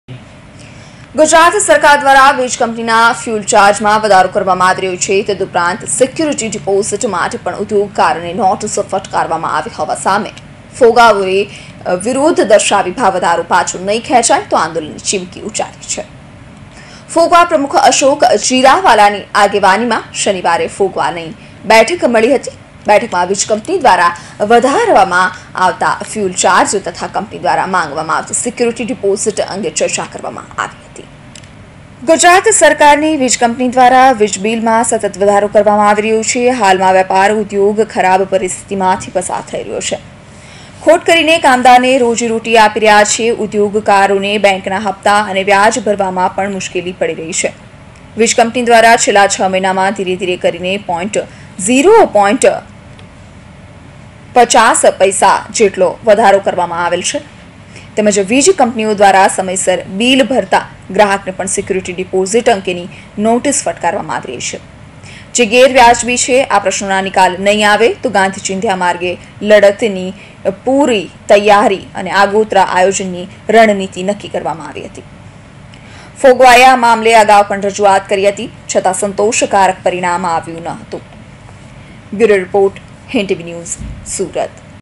વીજ કંપની ભાવવધારો પરત નહીં ખેંચે તો આંદોલન : ફોગવા
6 મહિનામાં ધીરે ધીરે 50 પૈસાનો વધારો કરી દેવાયો
સિક્યોરિટી ડિપોઝીટ માટે પણ નોટિસ